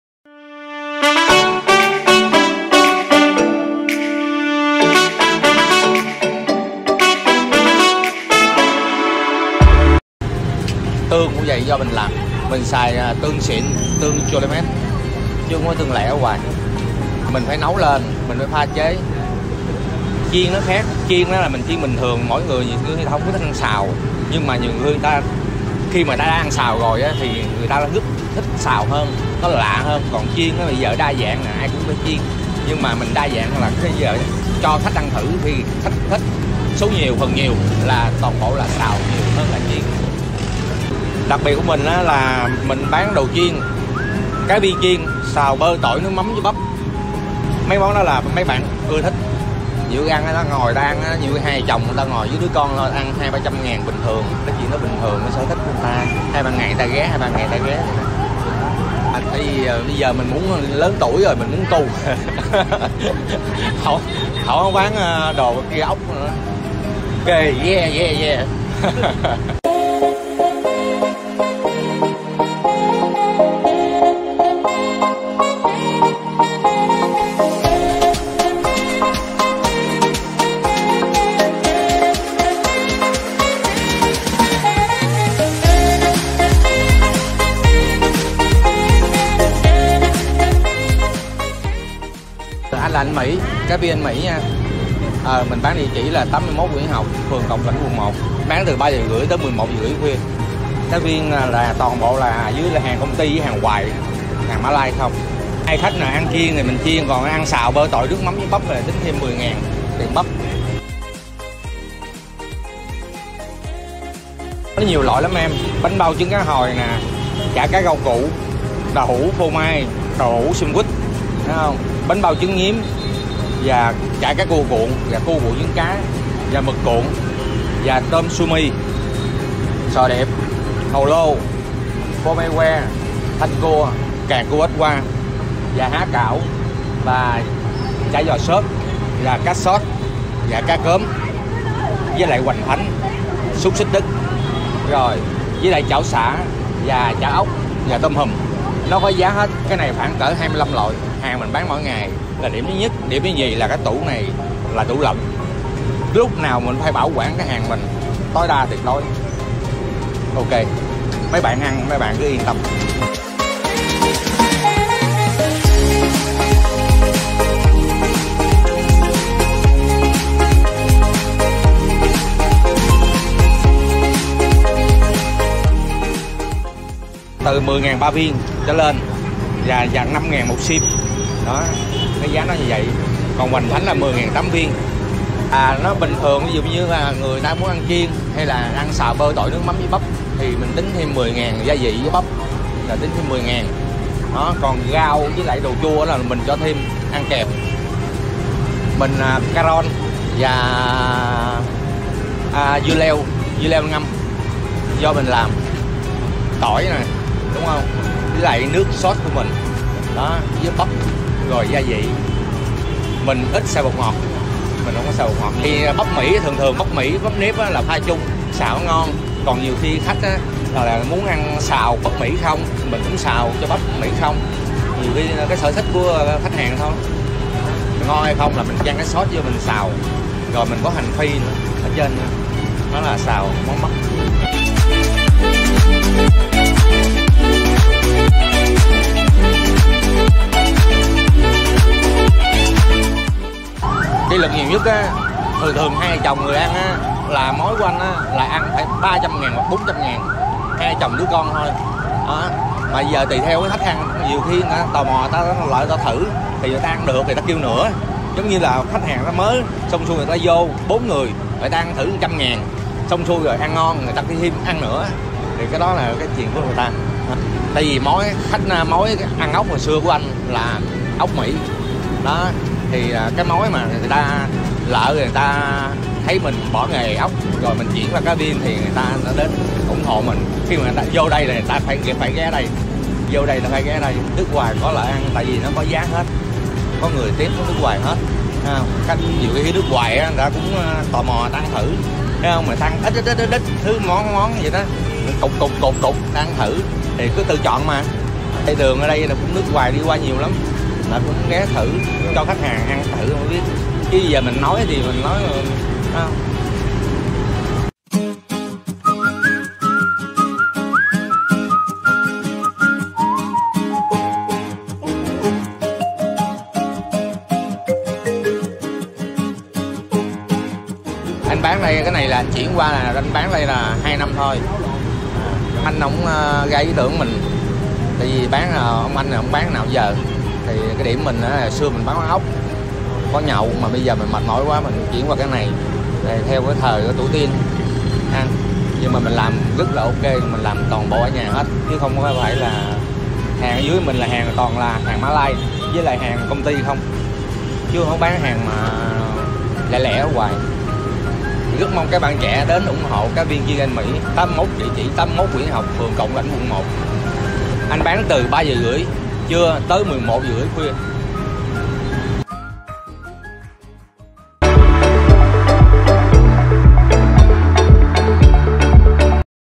Buffet cá viên chiên ăn thả ga, giá rẻ bất ngờ
BUFFET CÁ VIÊN CHIÊN XÀO BẮP GIÁ RẺ BẤT NGỜ NGAY QUẬN 1
Ăn tẹt ga tha hồ chọn BUFFET CÁ VIÊN CHIÊN XÀO BẮP giá rẻ bất ngờ

Biến tấu mới gây nghiện khó mà cai được nè. cá viên chiên tự chọn topping như buffet xào bắp thơn ngon nhịn hông nổi luôn, càng ăn càng ghiền í. ai chưa thử ghé liền nè